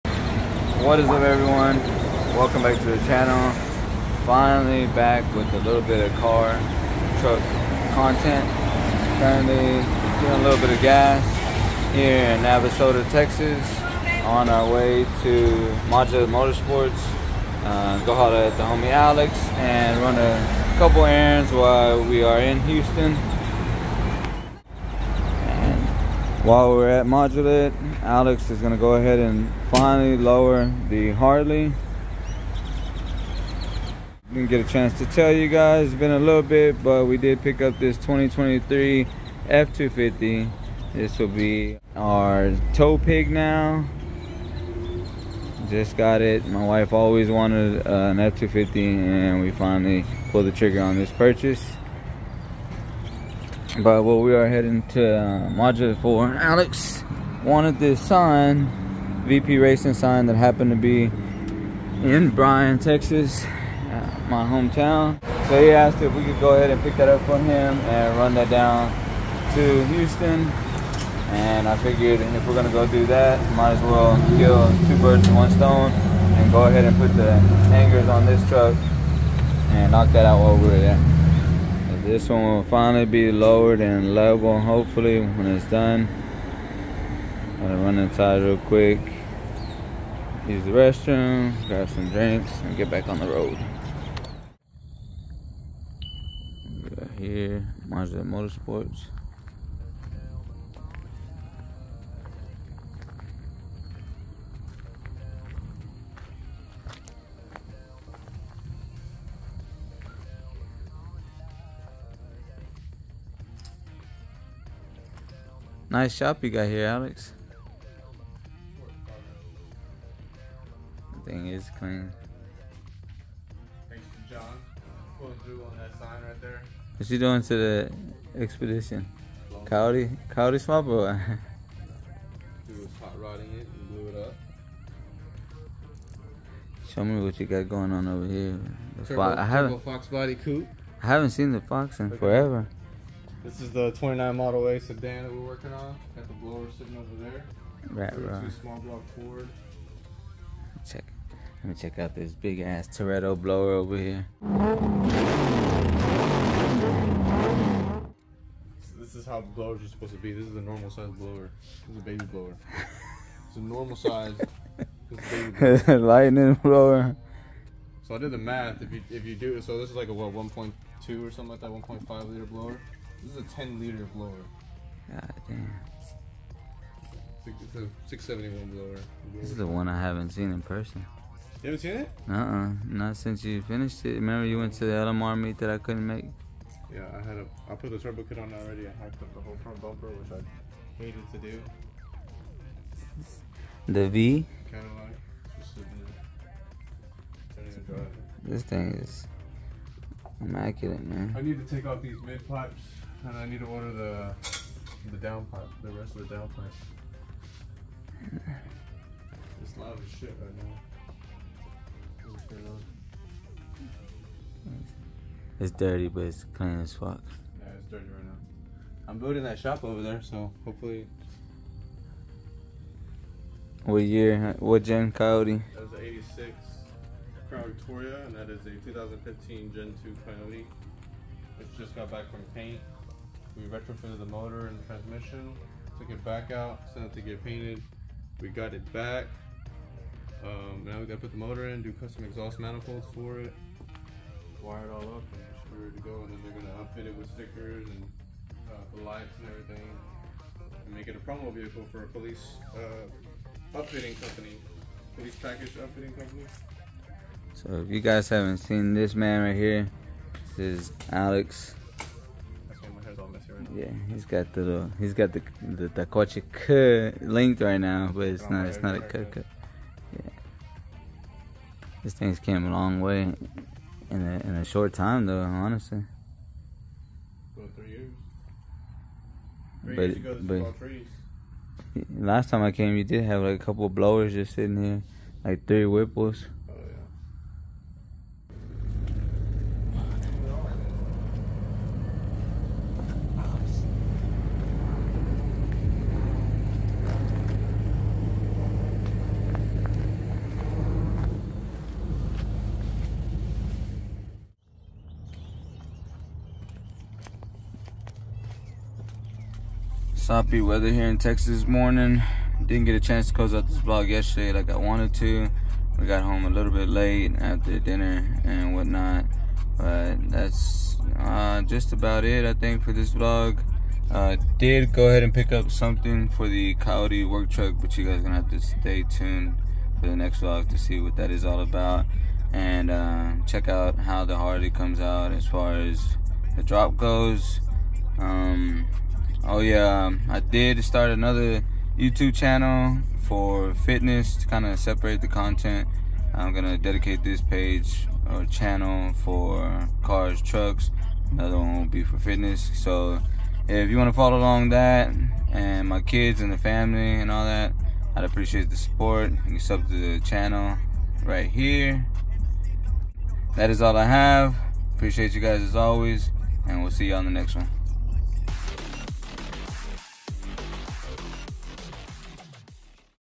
Fixing my 2003 F150 Harley | 🛻 NEW TRUCK & NEW CHANNEL 📺
Back with some fresh truck content! This vlog we hit up Modulate Motorsports in Houston Texas 🤘🏼 to get the supercharged Harley right and take care of some other things while we're in town. Check it out, like, SUB, share and let me know what you guys think in the comments!

🅽🅴🆆 channel for fitness, training and nutrition based content 👇🏼